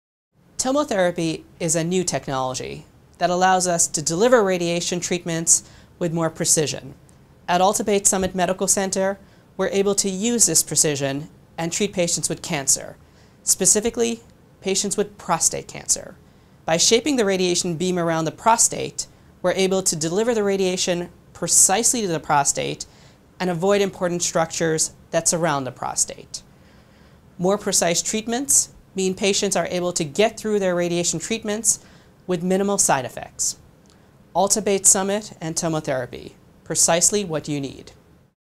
Tomotherapy for Prostate Cancer Treatment
Tomotherapy is a new technology that allows us to deliver radiation treatments with more precision.  At Alta Bates Summit Medical Center we are able to use this precision and treat patients with cancer, specifically, patients with prostate cancer.  By shaping the radiation beam around the prostate we are able to deliver the radiation precisely to the prostate and avoid important structures that surround the prostate.  More precise treatments mean patients are able to get through their radiation treatments with minimal side affects.  Alta Bates Summit and Tomotherapy, precisely what you need.